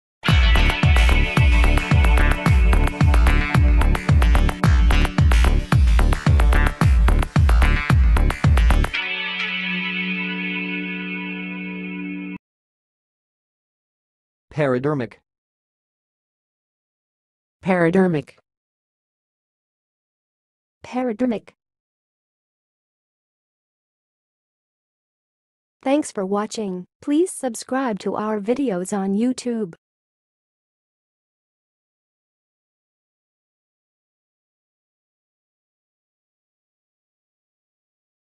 How to say "peridermic"! (High Quality Voices)
Watch in this video how to say and pronounce "peridermic"!